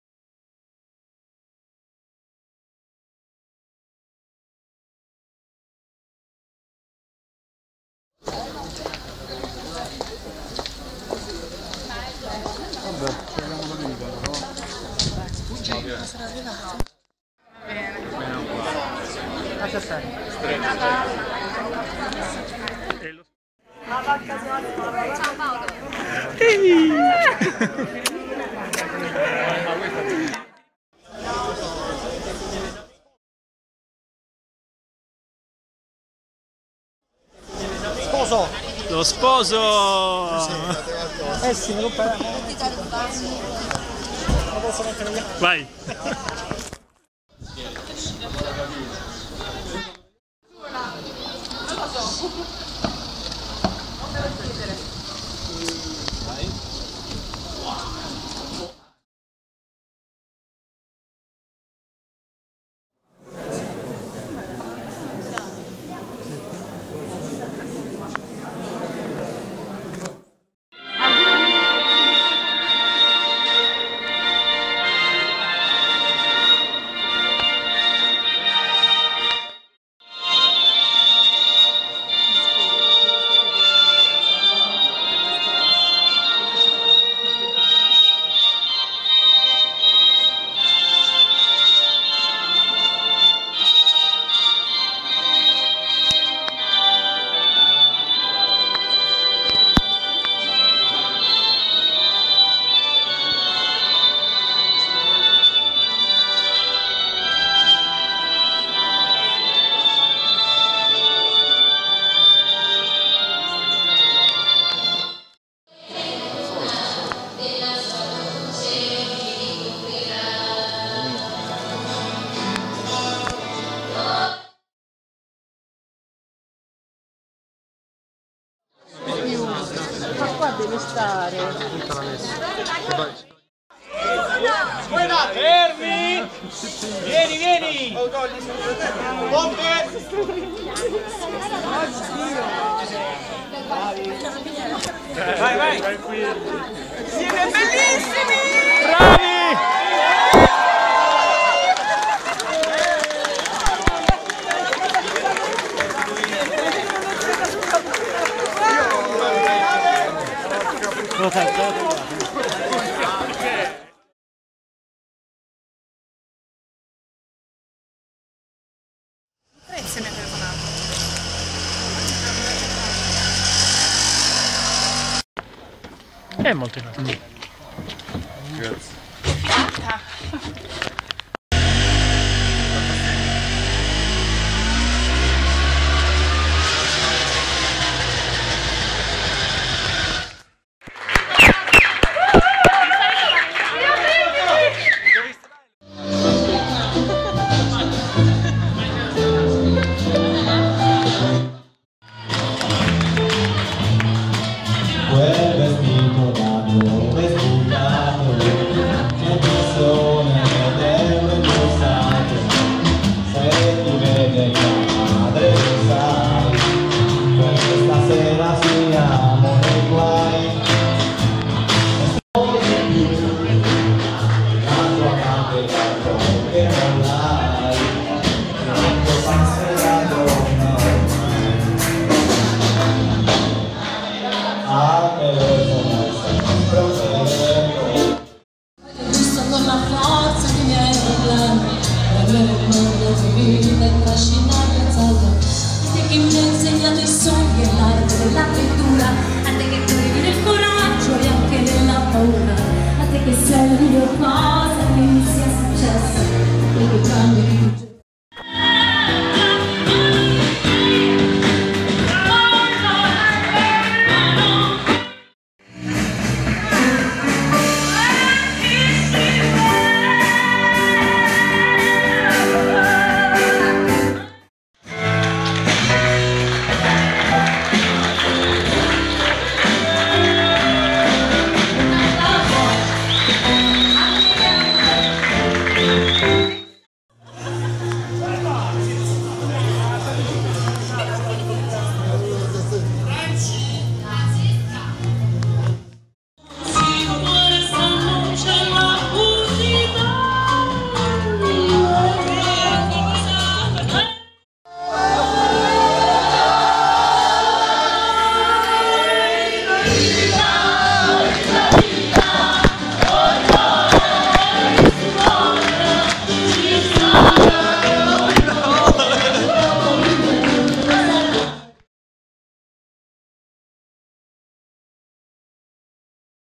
Miriam e Giuseppe Matrimonio
video-sintesi del matrimonio tra Miriam e Giuseppe  (Chiaiano - 07/06/2008)